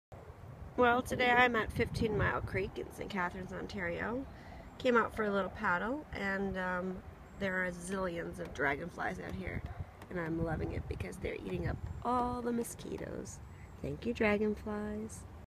kayaking 16 mile creek dragonflies july 12 2015
Kayaking on 16 Mile creek in St. Catharines, Ontario, July 12, 2015. Watching the dragonflies eat the mosquitos!!!